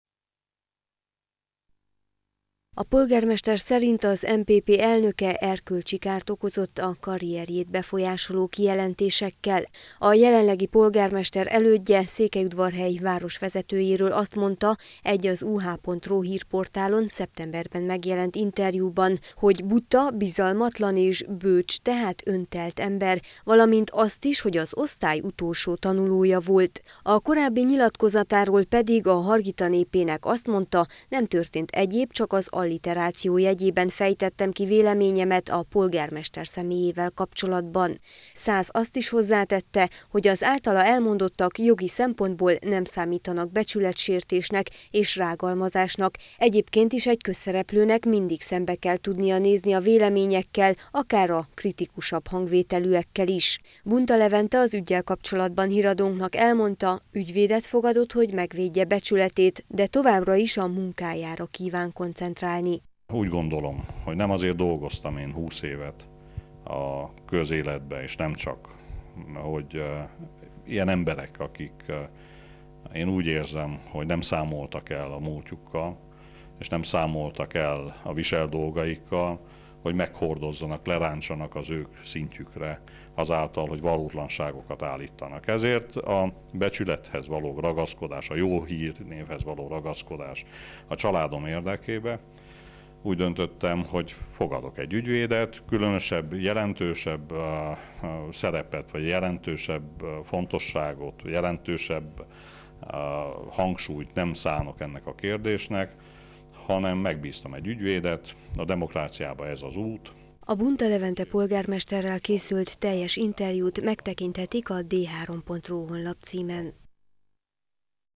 Bunta rágalmazásért pereli Szászt
Beperelte rágalmazásért Bunta Levente székelyudvarhelyi polgármester Szász Jenőt, az MPP elnökét, városi tanácsost. Szász február 2-ra kapott idézést a bíróságra, Bunta 10 000 lej kártérítést követel egy székelyudvarhelyi hírportálon megjelent interjúban elhangzottakért, továbbá arra is kötelezné Szászt, hogy a bírósági határozatot hozza nyilvánosságra -- saját költségén -- két helyi újságban.